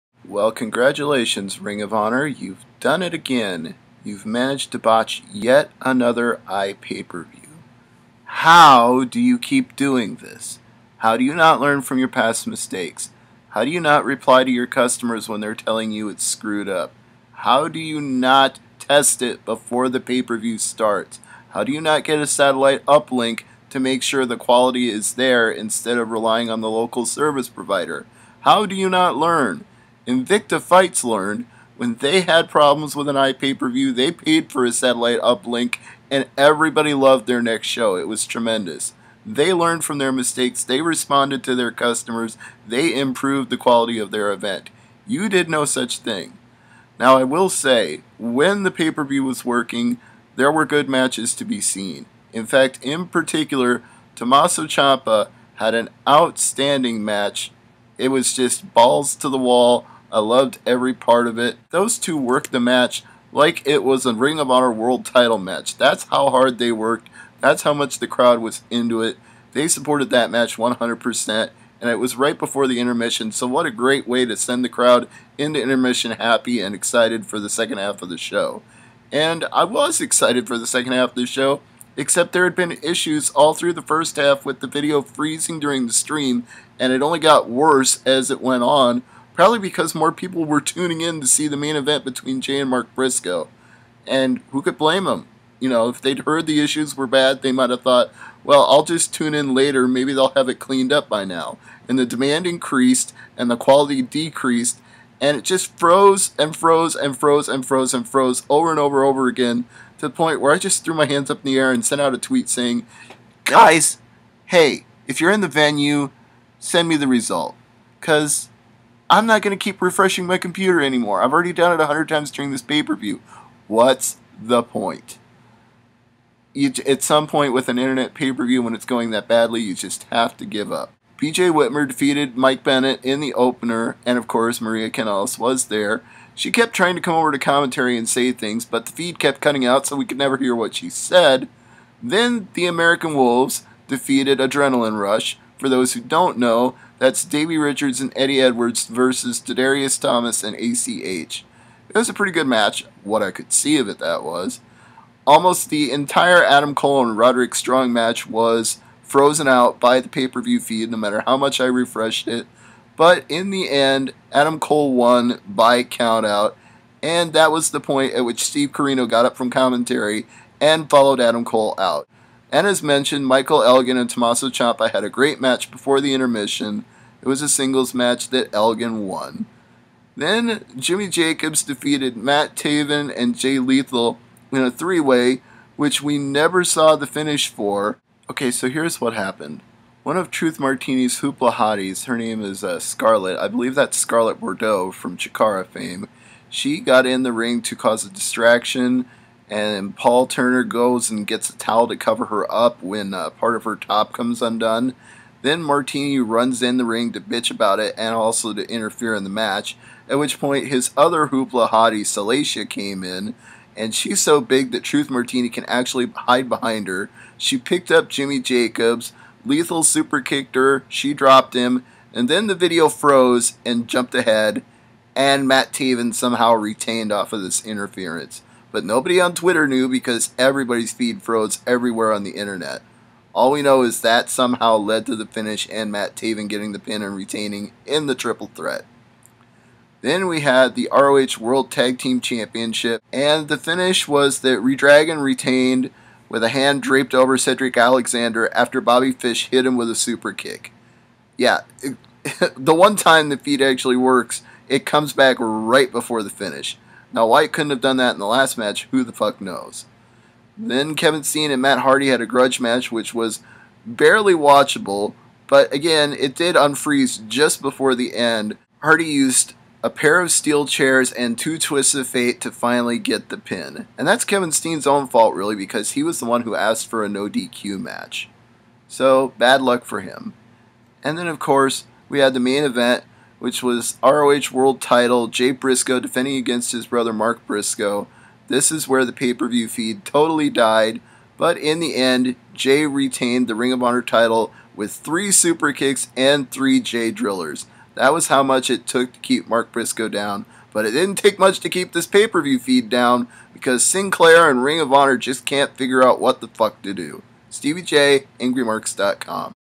ROH Best in the World 2013 Recap - An Internet PPV Debacle
Congratulations Ring of Honor, you've done it again. You never learn do you? It's all false promises with you ROH. You say you learned from your mistakes - yet you don't. You say you've tested the servers to make sure they can handle it - but then the feed freezes and buffers constantly. We are the fools for believing you and giving you our money. Jay Briscoe retained the ROH World Title over his brother Mark Briscoe in an essentially unwatchable main event. For my money (most of which was wasted) the main event was actually Tommaso Ciampa (@ProjectCiampa) vs. Michael Elgin (@ROHMichaelElgin), who wrestled like there was a title on the line in the last match before intermission. Everybody on this card worked really hard, it's just a shame that because Sinclair and ROH are so inept, most of it went unseen except by the people lucky enough to be live in the building.